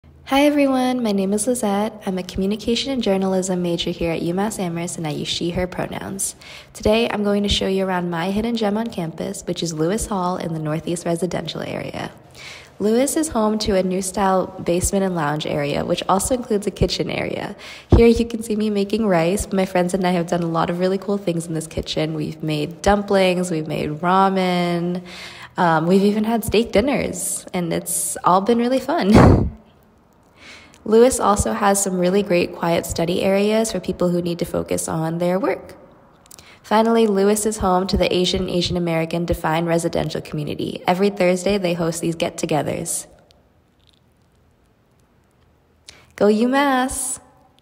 UMass Amherst Hidden Gems: Lewis Hall
- Hi everyone. My name is Lizette. I'm a communication and journalism major here at UMass Amherst and I use she/her pronouns. Today, I'm going to show you around my hidden gem on campus, which is Lewis Hall in the Northeast Residential Area. Lewis is home to a new style basement and lounge area, which also includes a kitchen area. Here, you can see me making rice. My friends and I have done a lot of really cool things in this kitchen. We've made dumplings. We've made ramen. We've even had steak dinners and it's all been really fun. Lewis also has some really great quiet study areas, for people who need to focus on their work. Finally, Lewis is home to the Asian/Asian American Defined Residential Community. Every Thursday, they host these get togethers. Go UMass!